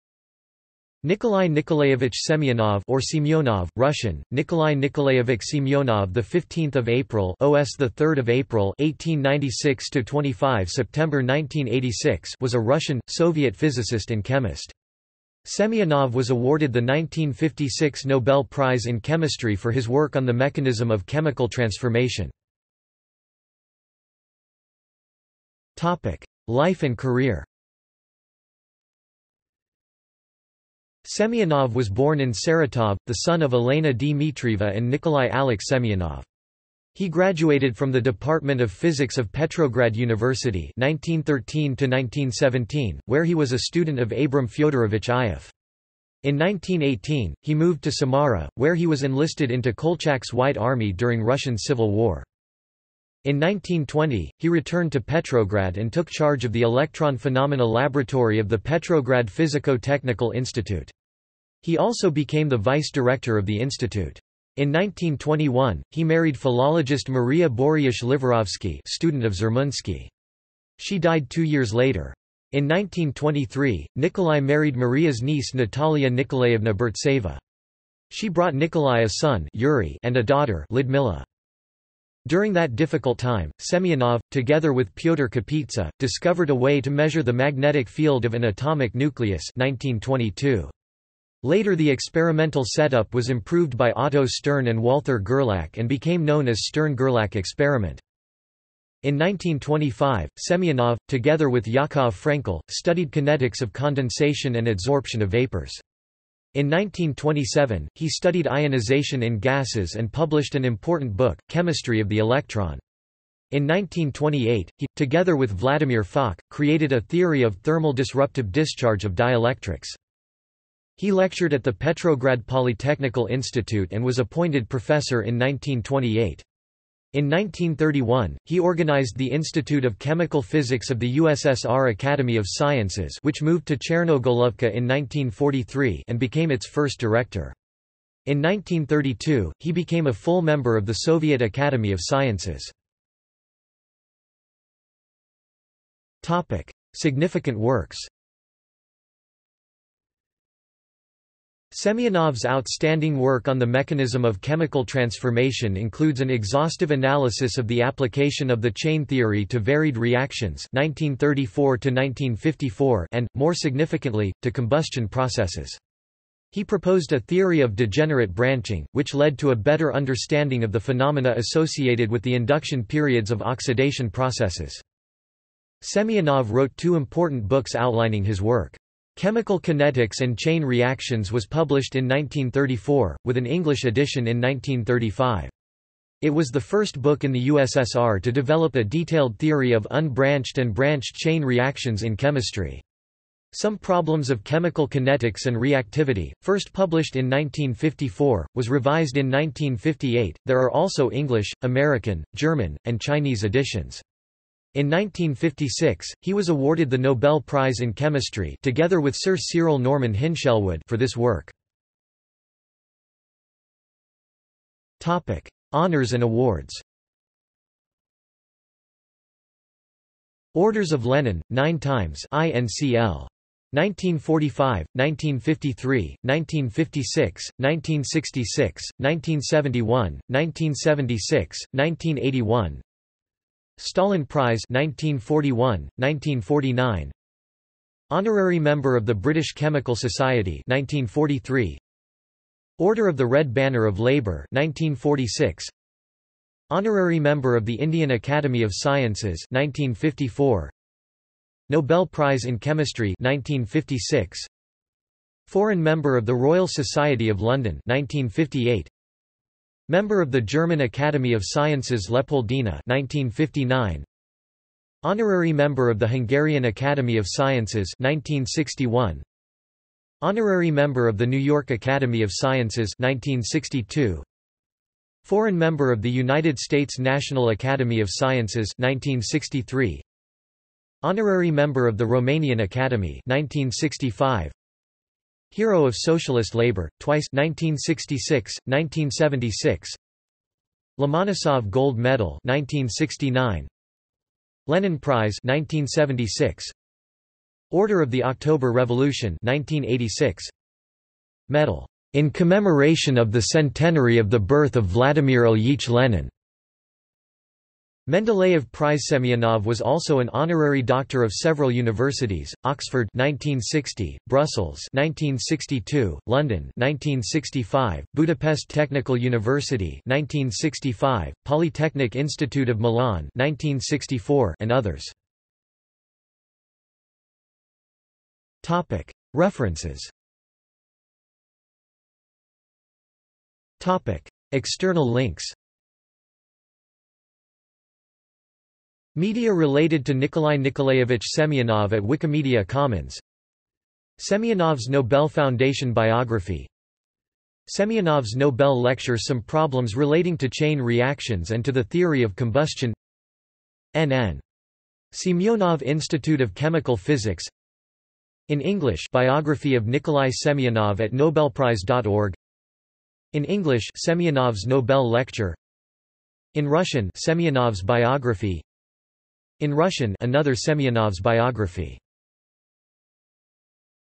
Nikolay Semyonov | Wikipedia audio article
This is an audio version of the Wikipedia Article:
Nikolay Semyonov

- Socrates

SUMMARY
Nikolay Nikolayevich Semyonov (or Semenov),  (Russian: Никола́й Никола́евич Семёнов; 15 April [O.S. 3 April] 1896 – 25 September 1986) was a Russian/Soviet physicist and chemist.  Semyonov was awarded the 1956 Nobel Prize in Chemistry for his work on the mechanism of chemical transformation.